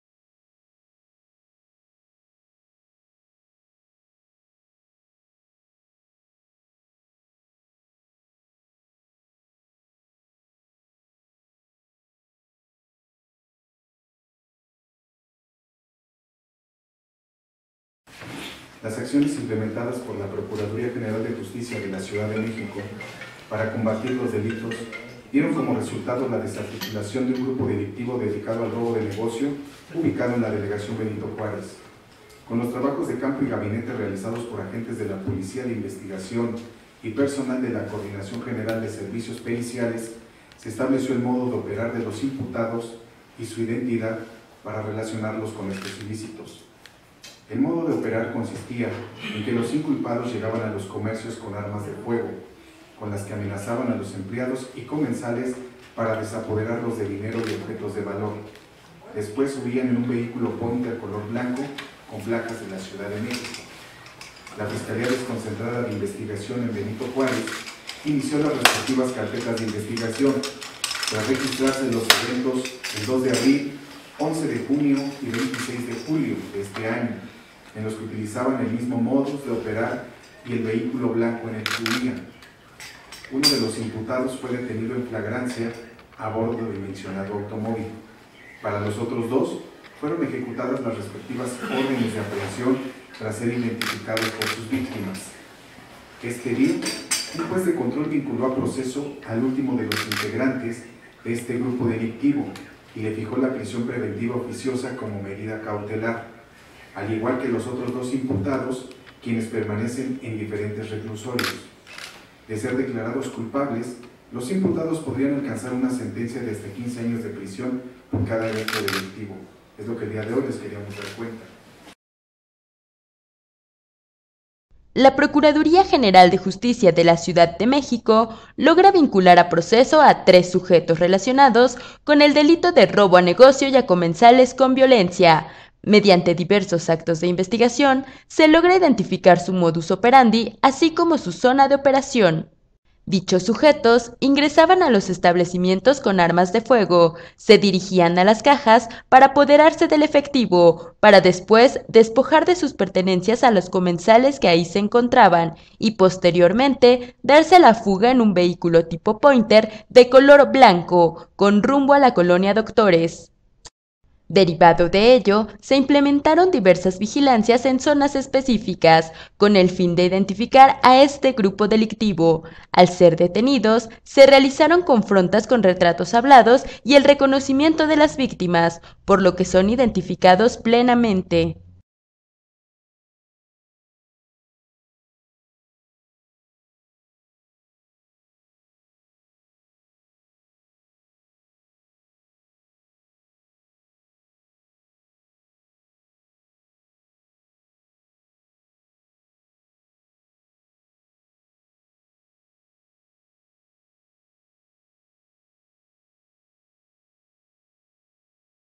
LA PGJCDMX DESARTICULÓ UNA ORGANIZACIÓN DELICTIVA DEDICADA AL ROBO DE NEGOCIOS EN BENITO JUÁREZ
*Fueron vinculados a proceso.
 *Están relacionados con tres ilícitos.